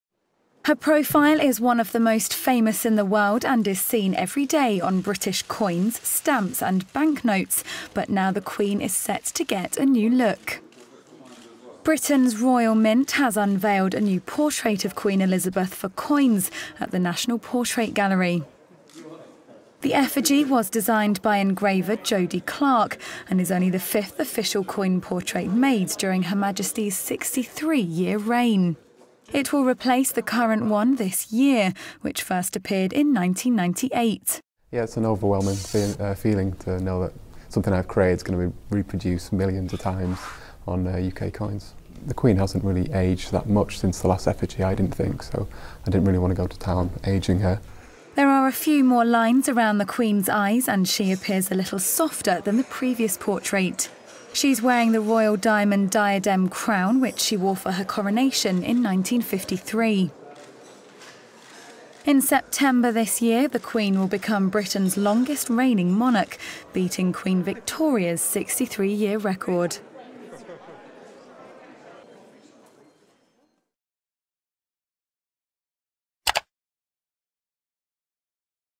New coinage portrait of the Queen is unveiled
Her profile is one of the most famous in the world and is seen every day on British coins but now the Queen is set to get a new look. Report by Claire Lomas.